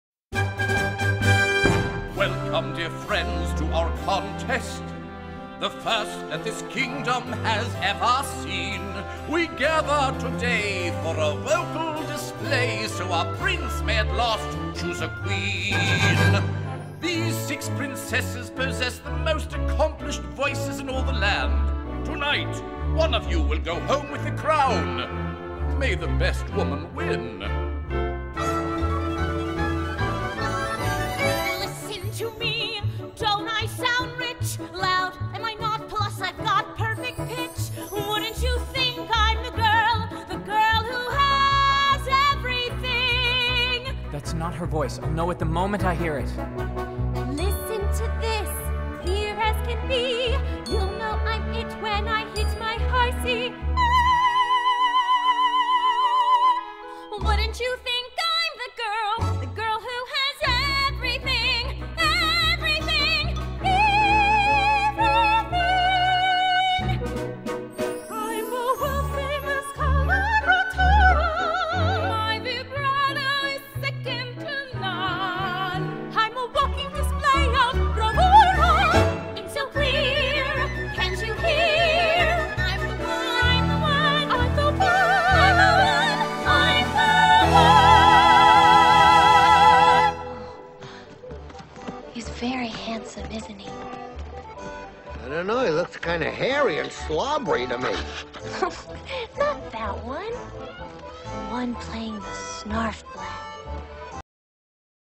The Contest
The princesses compete for Eric!